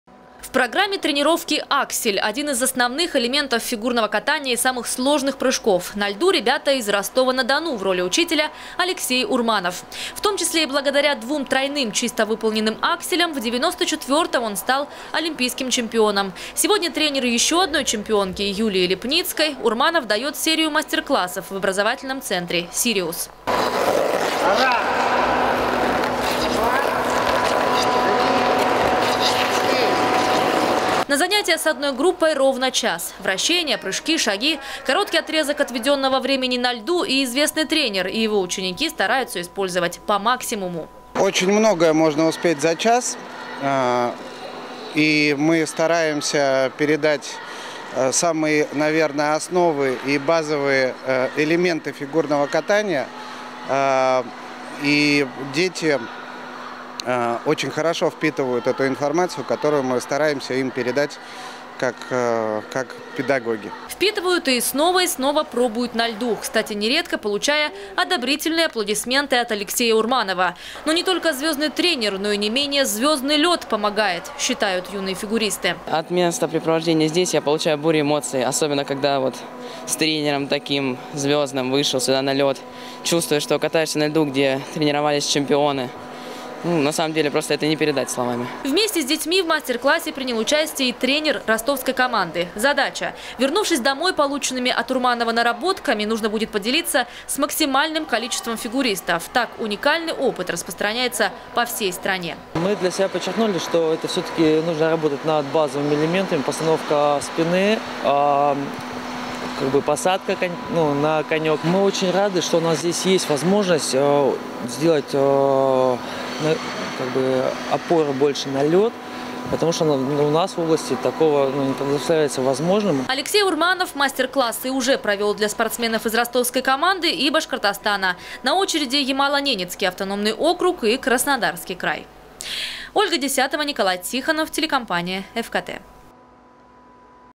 Известный тренер Алексей Урманов провел в Сочи мастер-класс. Новости. Эфкате.
Серию мастер-классов на льду от олимпийского чемпиона, тренера Юлии Липницкой организовали для участников апрельской смены образовательного центра «Сириус».

В программе тренировки аксель — один из основных элементов фигурного катания и самых сложных прыжков. На льду — ребята из Ростова-на-Дону. В роли учителя — Алексей Урманов. В том числе и благодаря двум тройным чисто выполненным акселям в 94-ом он стал олимпийским чемпионом. Сегодня тренер ещё одной чемпионки — Юлии Липницкой, Урманов даёт серию мастер-классов в образовательном центре «Сириус».

На занятие с одной группой ровно час. Вращения, прыжки, шаги — короткий отрезок отведенного времени на льду и известный тренер, и его ученики стараются использовать по максимуму.

Сочи. Новости.
Телеканал Эфкате.